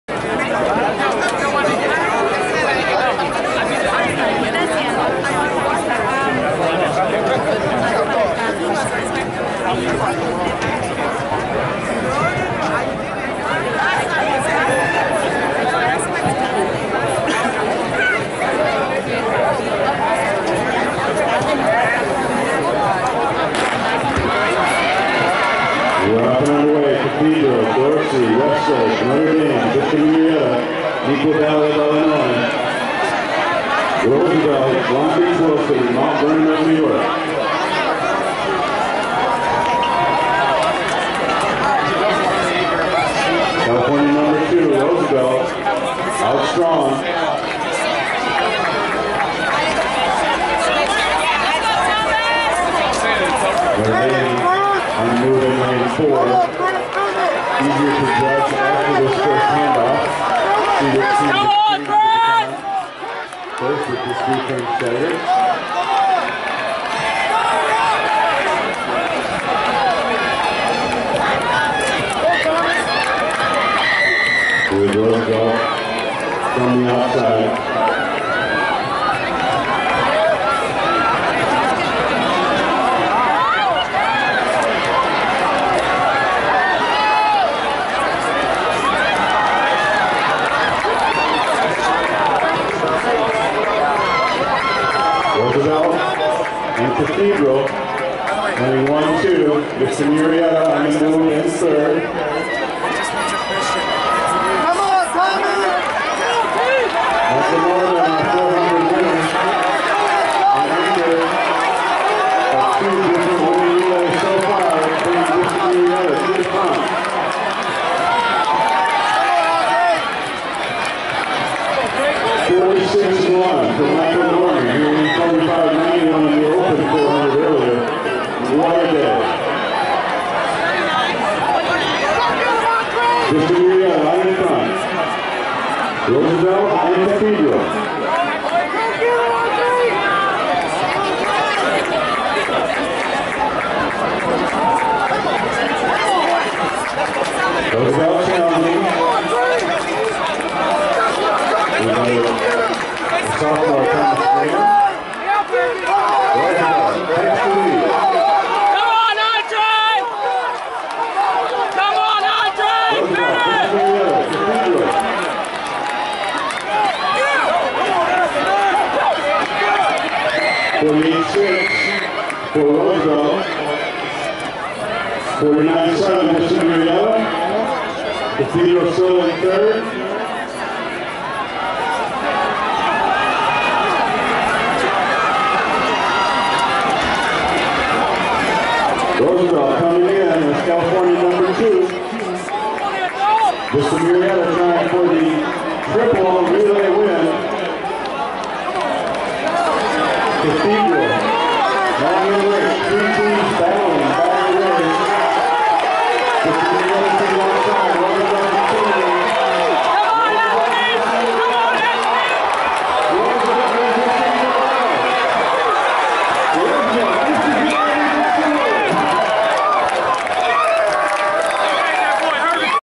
Arcadia Invitational 4x400 Men Invitational 2015
April 11, 2015 Notre Dame Sherman Oaks 3:18.23